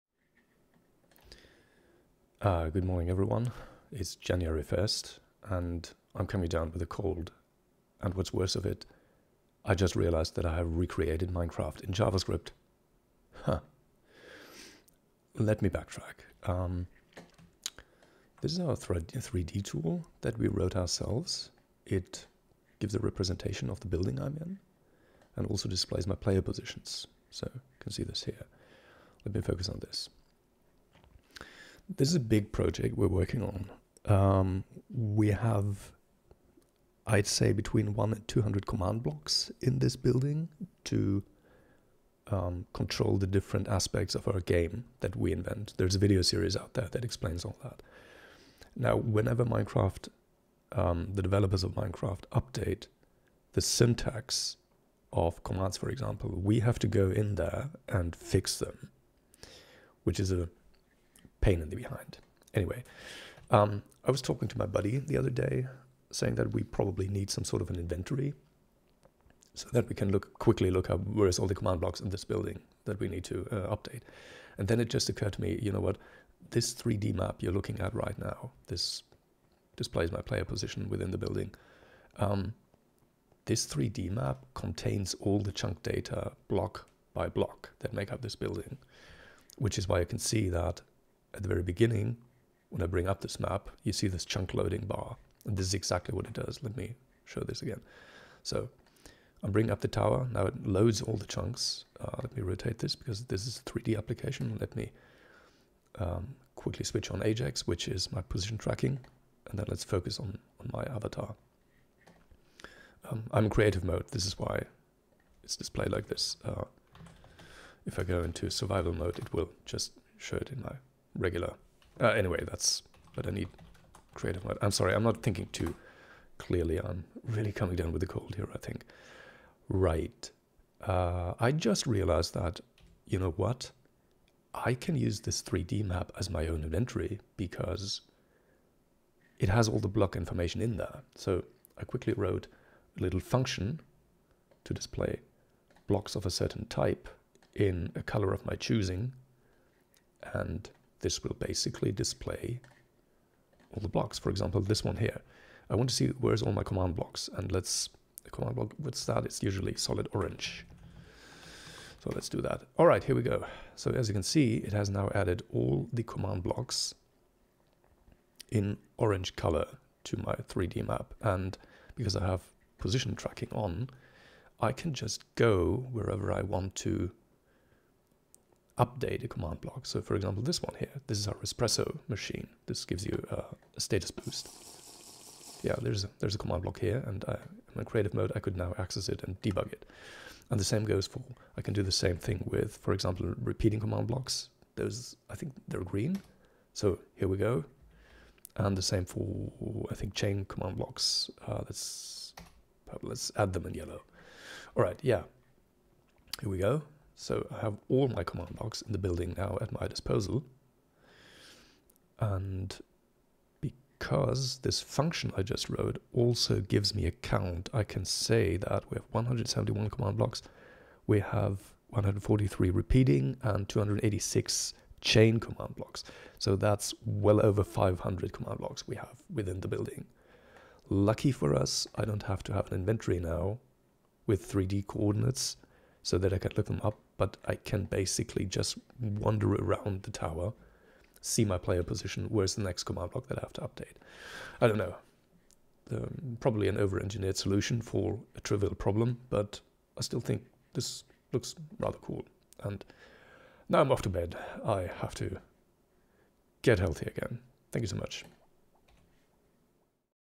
I accidentally recreated Minecraft in Javascript
Our custom-built 3D application is capable of displaying Minecraft-native chunk data block by block.

We can use this e.g. for debugging command blocks dispersed throughout our large tactical map builds for our "Tower Heist" minigame.

== GitHub repository of the V.I.T.A.L. web-application ==

== GitHub repository of the whimc player position tracking plugin ==